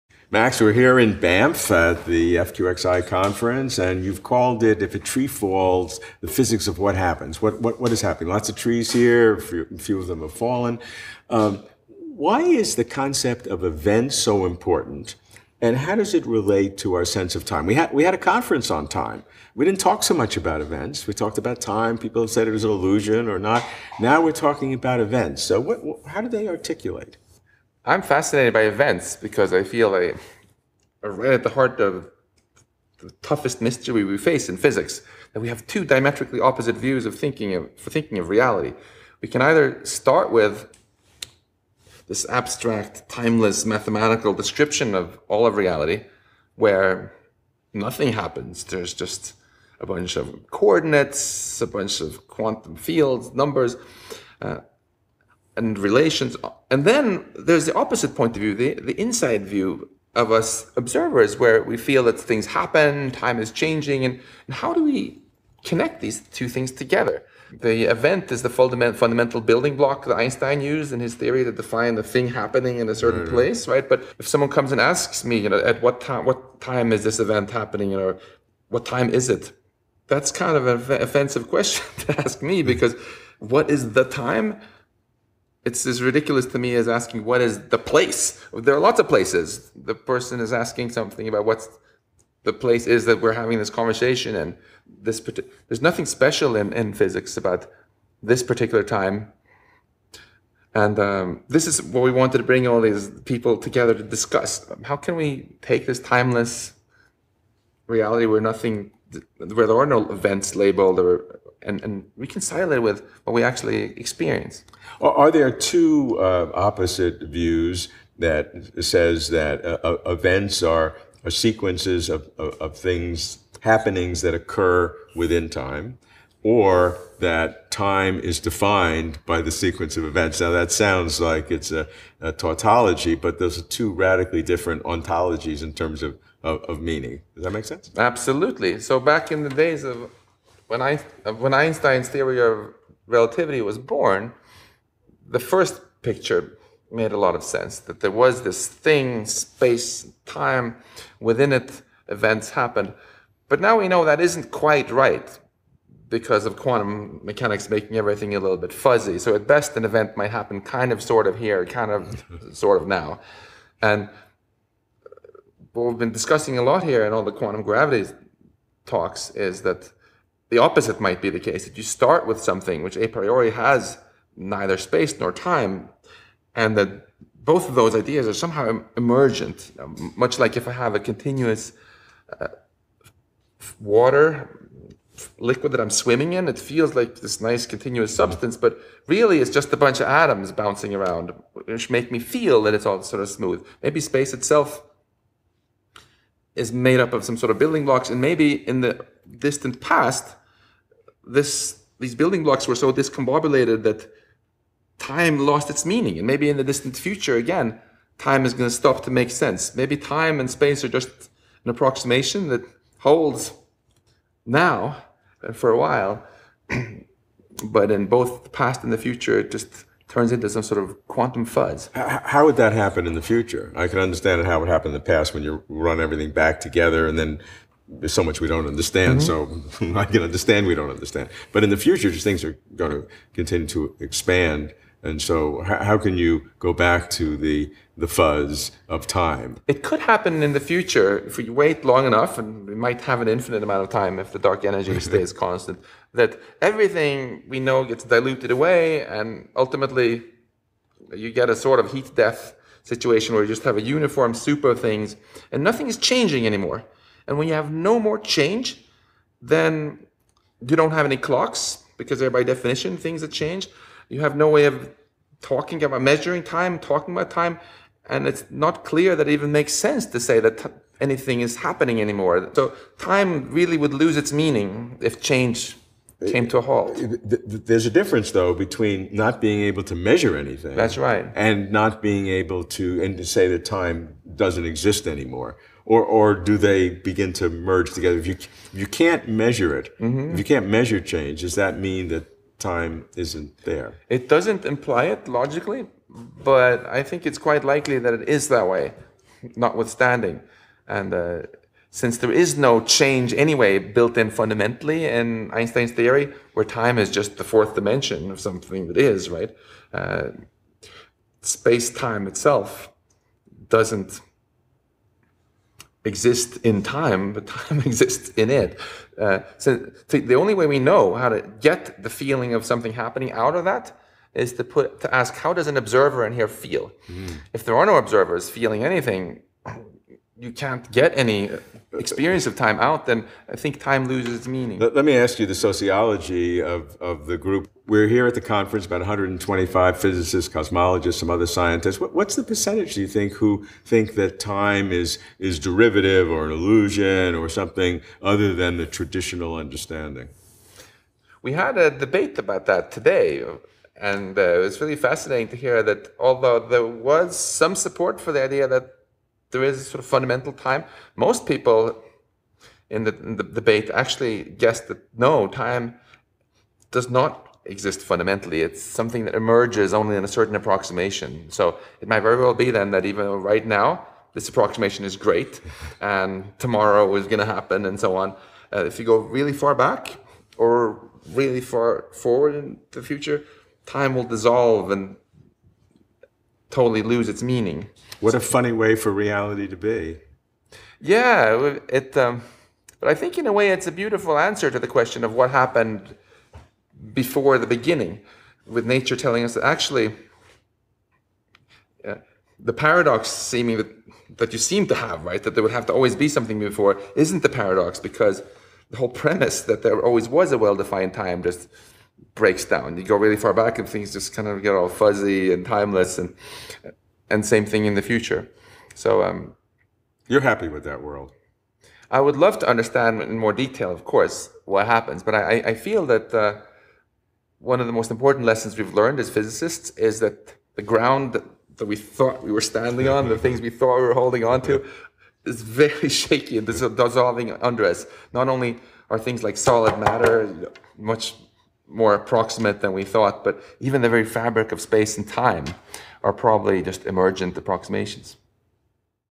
Max Tegmark - Events and the Nature of Time
Are events measured by time? Or is time created by sequences of events? Which is more fundamental, events or time? Worse, we may get different answers from quantum mechanics and general relativity.